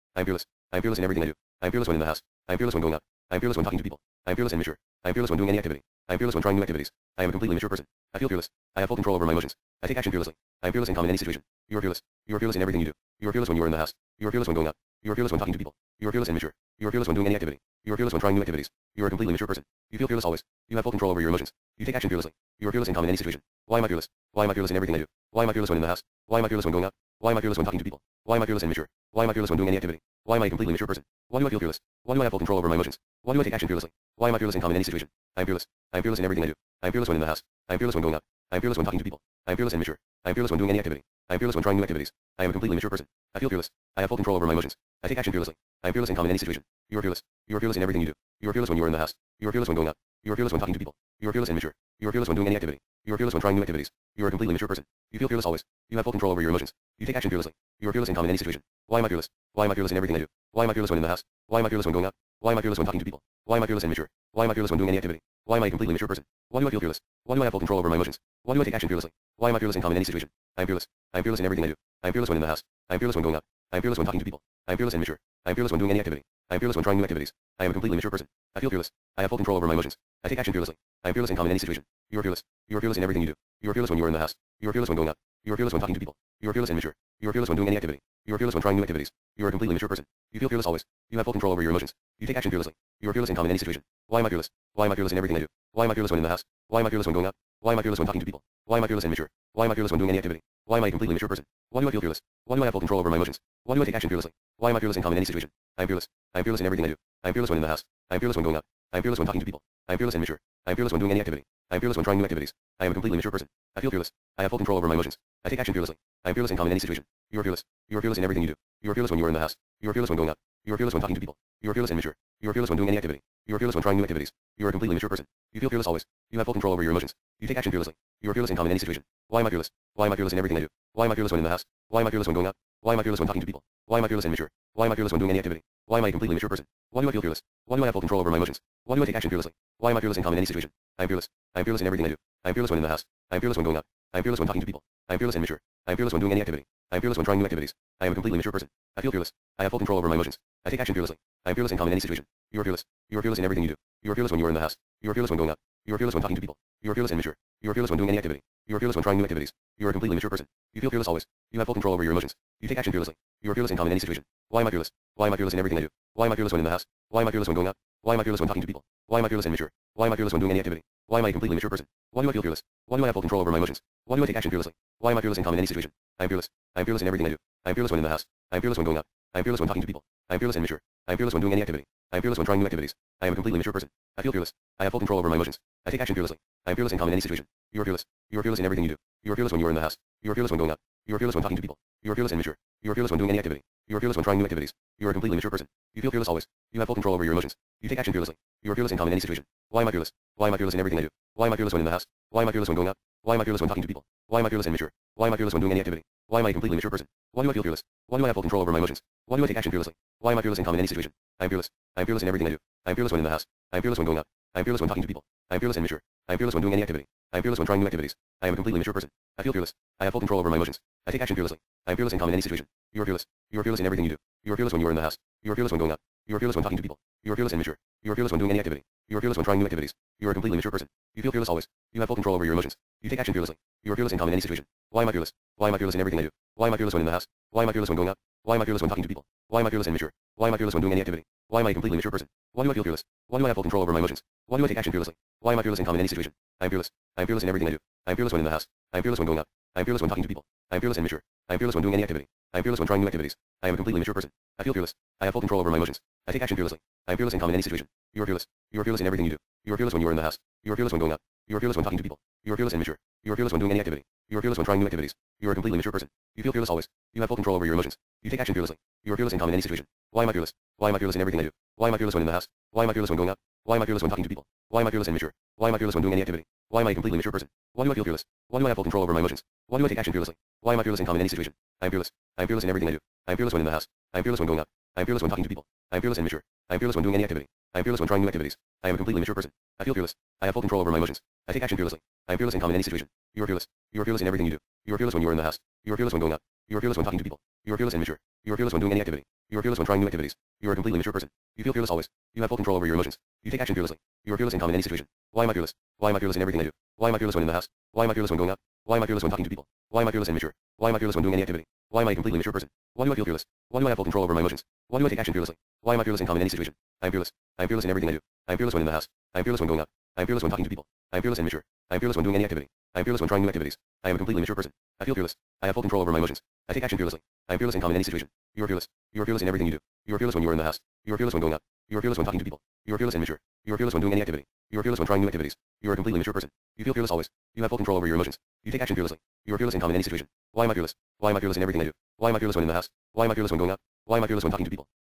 fearless (affirmations and afformations only)
yep no music lol

AFFIRMATIONS AND AFFORMATIONS LIST:

I am fearless
i am fearless in everything i do
i am fearless when in the house
i am fearless when going out
i am fearless when talking to people
i am fearless and mature
i am fearless when doing any activity
i am fearless when trying new activities
i am a completely mature person
i feel fearless
i have full control over my emotions
i take action fearlessly
i am fearless and calm in any situation
you are fearless
you are fearless in everything you do
you are fearless when you are in the house
you are fearless when going out
you are fearless when talking to people
you are fearless and mature
you are fearless when doing any activity
you are fearless when trying new activities
you are a completely mature person
you feel fearless always
you have full control over your emotions
you take action fearlessly
you are fearless and calm in any situation
why am i fearless?
why am i fearless in everything i do
why am i fearless when in the house?
why am i fearless when going out?
why am i fearless when talking to people?
why am i fearless and mature?
why am i fearless when doing any activity?
why am i a completely mature person?
why do i feel fearless?
why do i have full control over my emotions?
why do i take action fearlessly?
why am i fearless and calm in any situation?